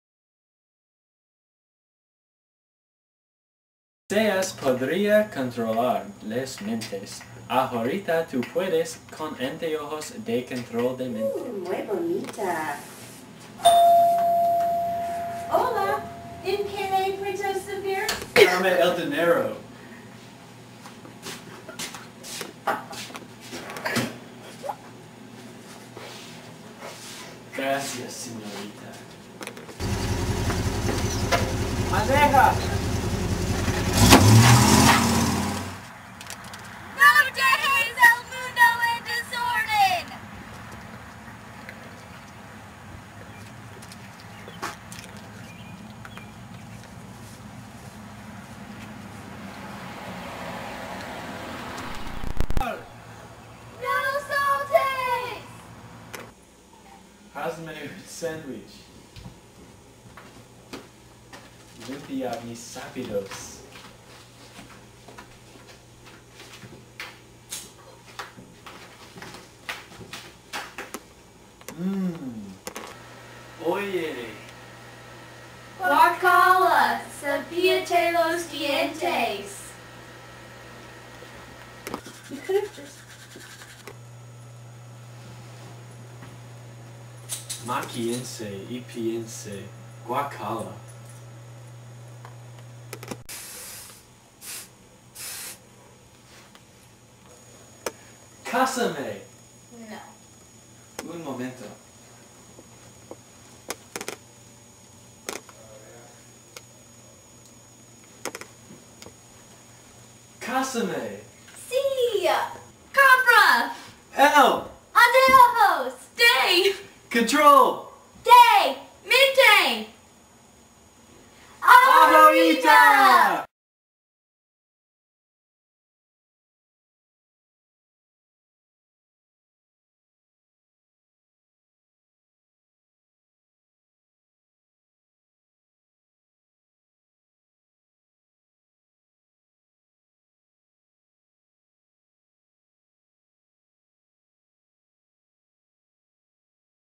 Staley Spanish 3 Project
Commercial for the mind control goggles.